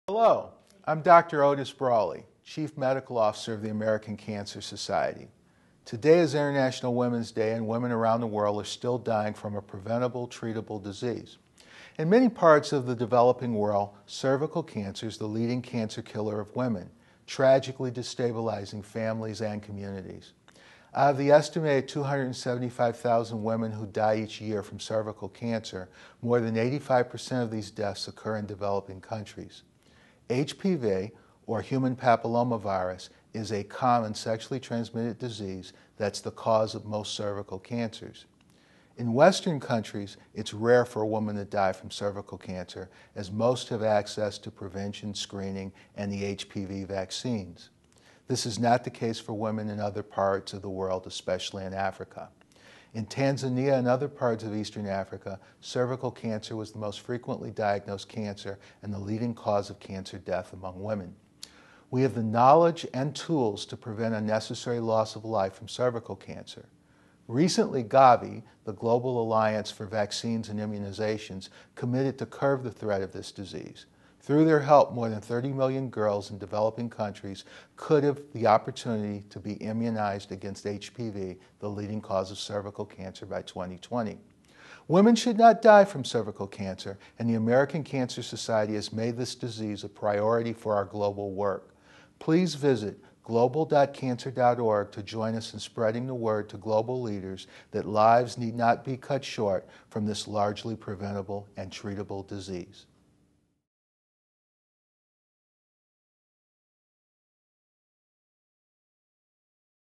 International Women's Day
Otis Brawley, MD, chief medical officer for the American Cancer Society, discusses the cervical cancer burden facing women around the world.

We have the knowledge and tools to prevent unnecessary loss of life from cervical cancer.